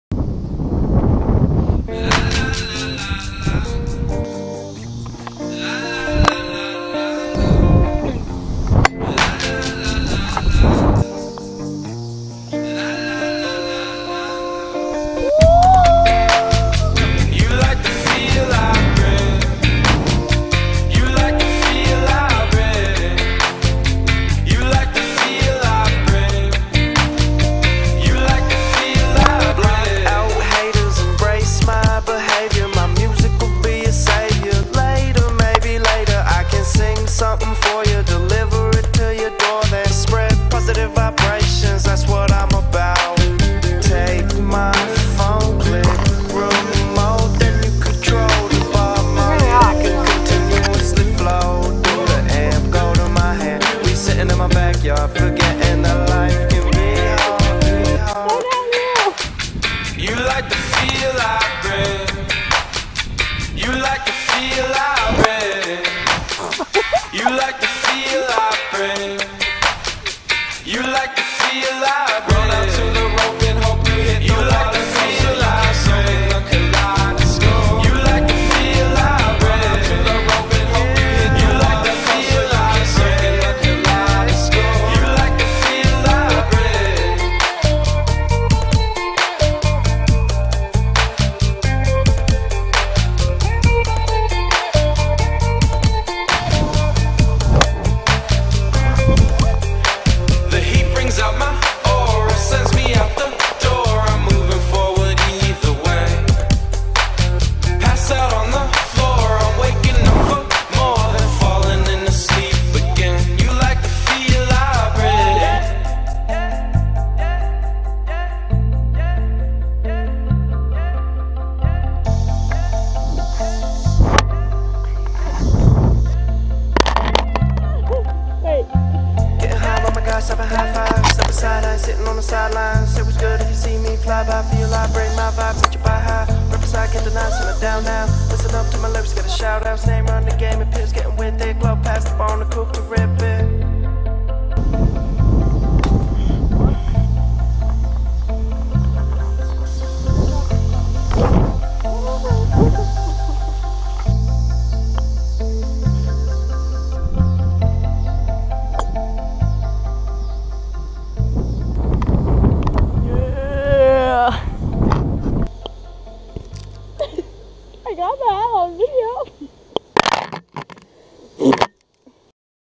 Attempt at a Golf Day Gopro
We ain't good at golf but we can't say that we didn't try. Gopro's are fun